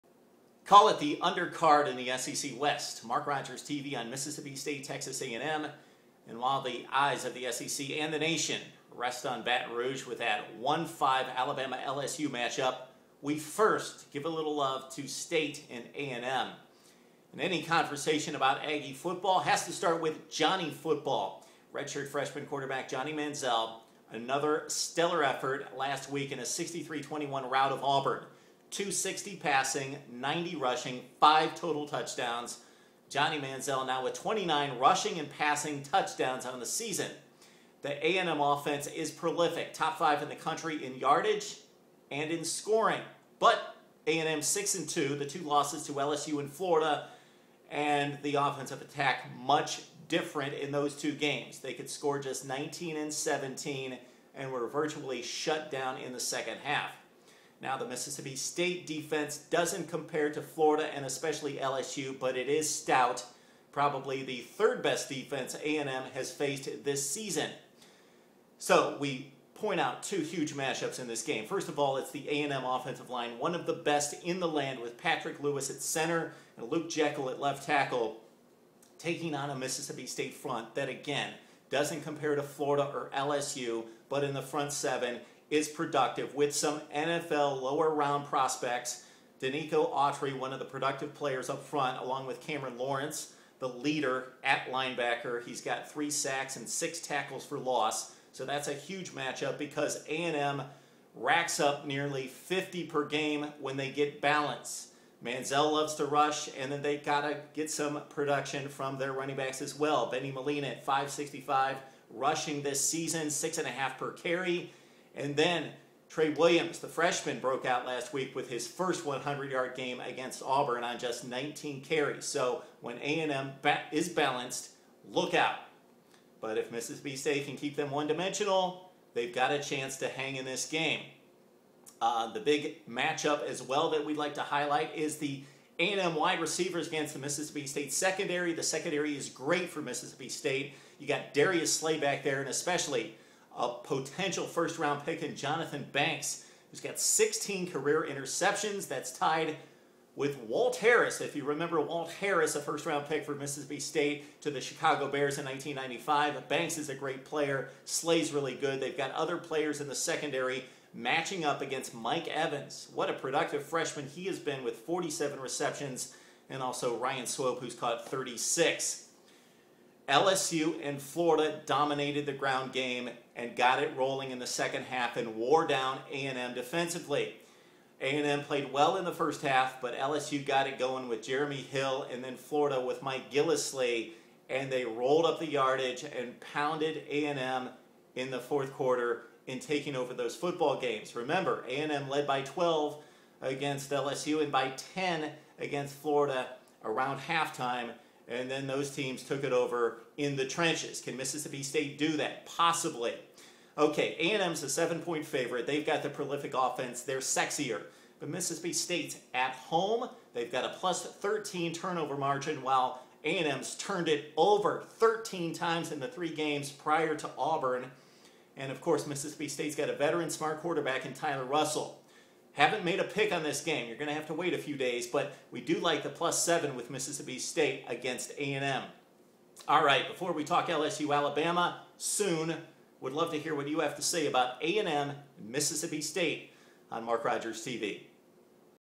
Texas A&M Aggies - Mississippi State Bulldogs Preview / Johnny Manziel, Tyler Russell
MarkRogersTV previews Mississippi State vs Texas A&M